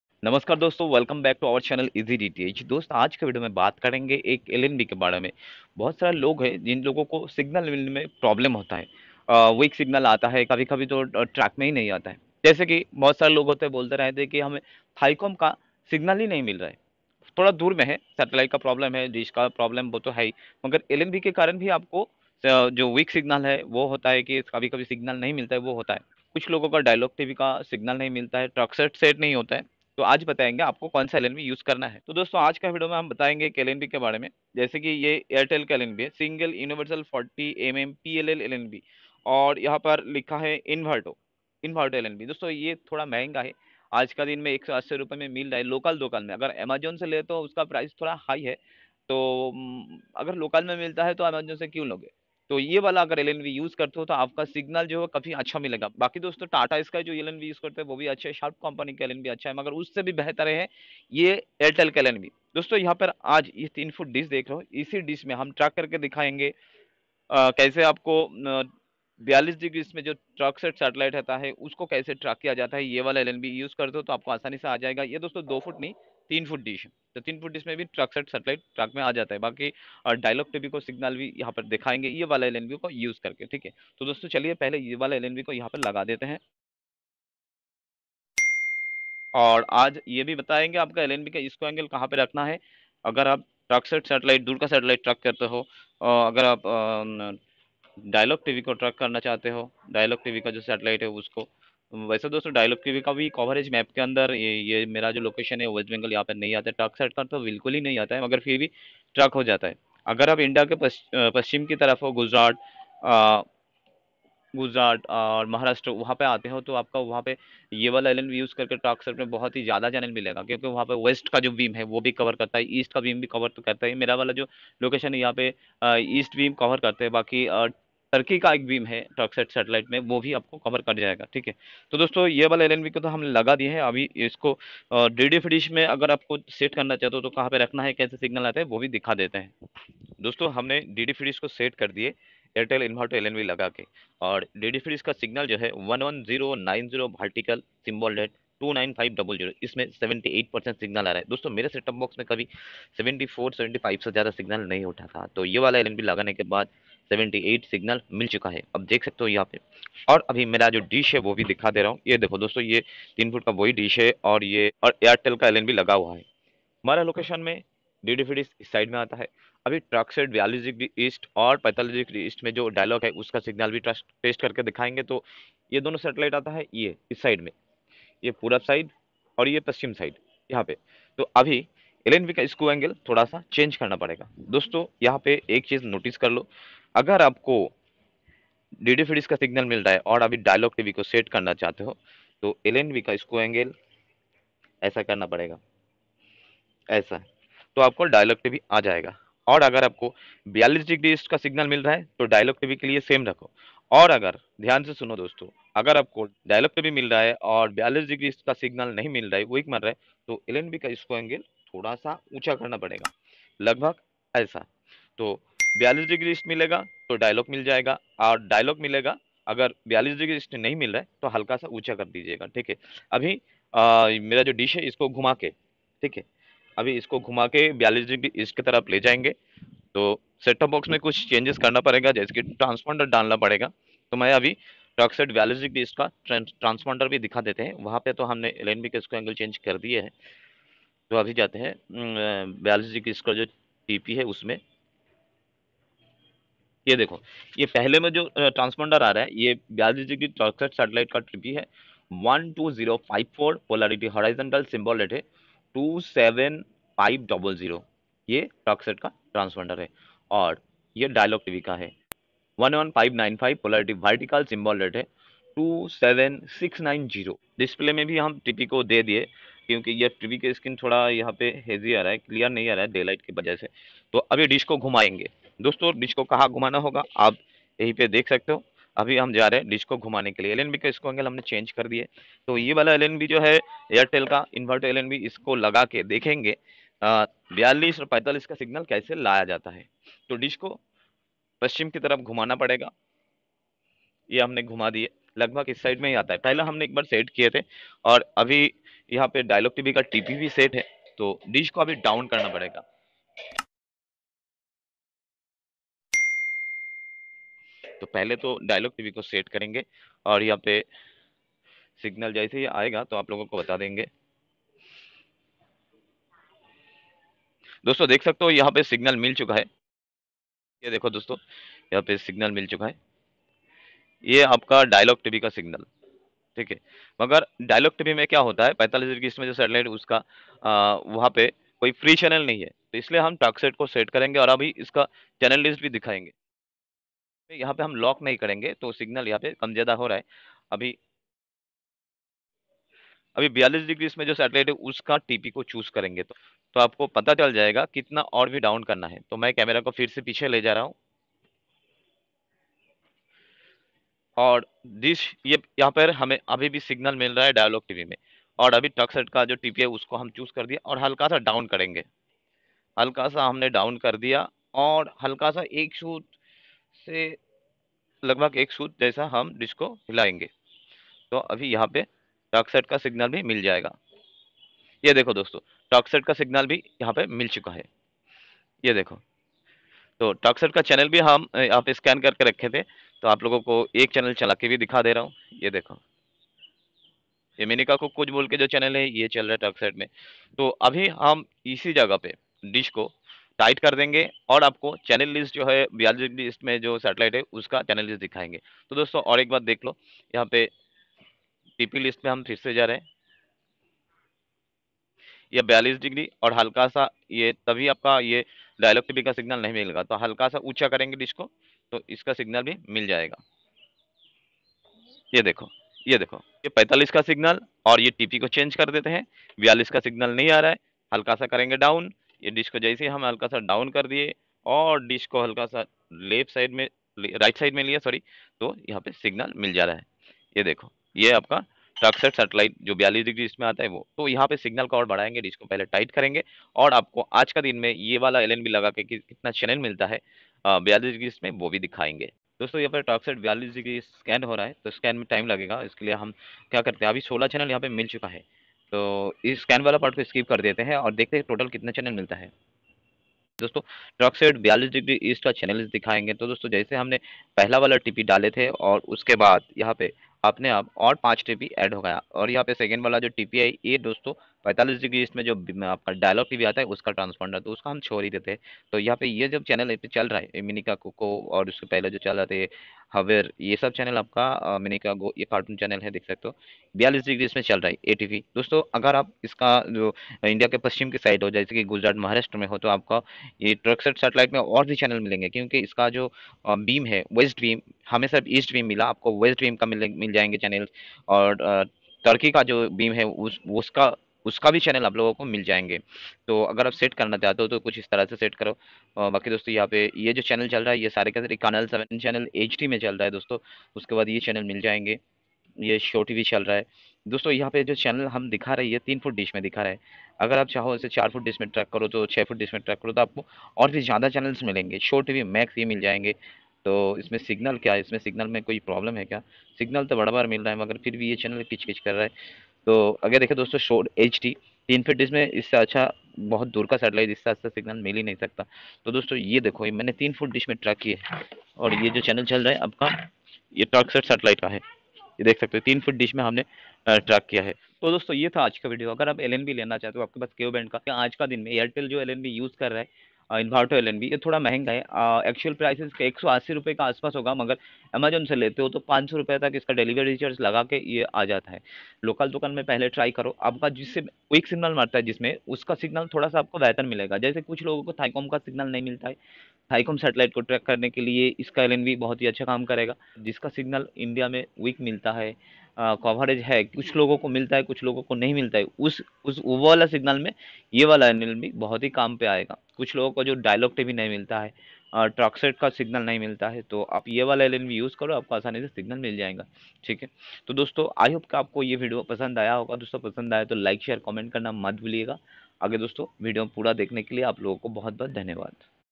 Get More Signal using Ku Band Airtel Inverto PLL LNB. Live signal test @42°E & 45°E.
Best Ku Band LNB for low/weak signal.

 Airtel Single Universal 40mm PLL Inverto LNB
ARTL-SNS411-0OPRO-OPN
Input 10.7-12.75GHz
Output: 950-2150MHz
LO: 9.75/10.6GHZ

If are getting weak signal then use this INVERTO LNB for better signal. In this Video I have shown live test traking.

Thanks a lot from
"Easy DTH"
Only Whatsapp message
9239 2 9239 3